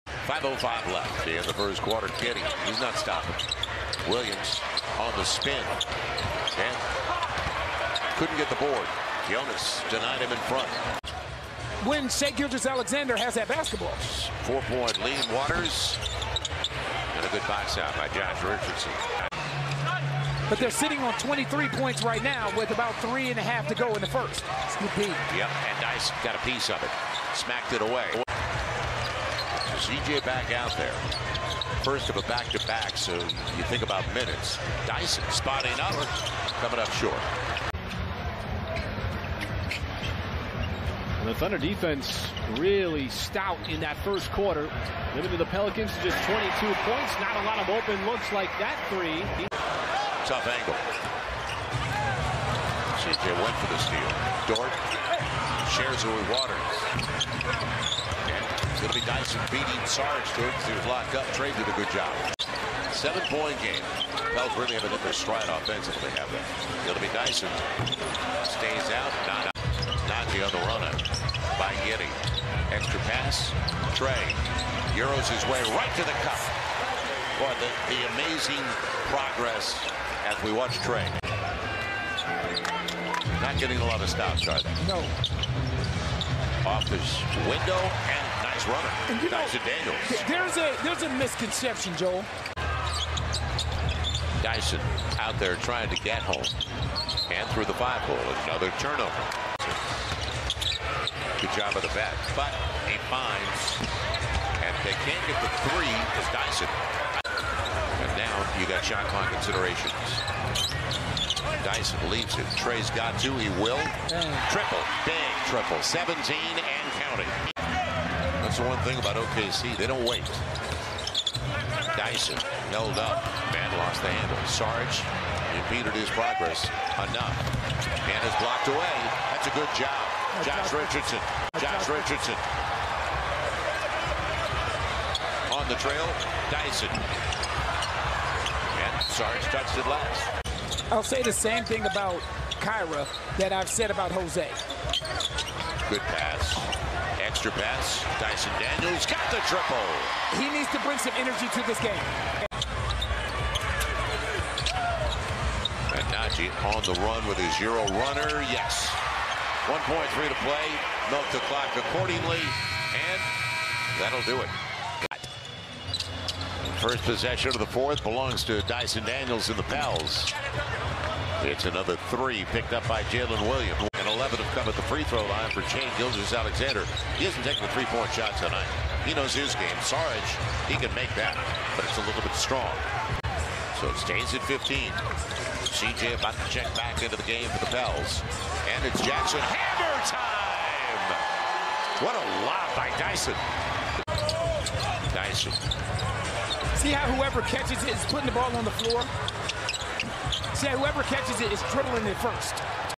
Dyson Daniels 6 REB: All Possessions (2023-03-11)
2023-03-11 New Orleans Pelicans vs Oklahoma City Thunder: Player Highlights Dyson Daniels
    5 PTS, 6 REB, 5 AST, 1 STL.
        1 of 5 from 3.
        2 of 7 FGs in 24:42 minutes.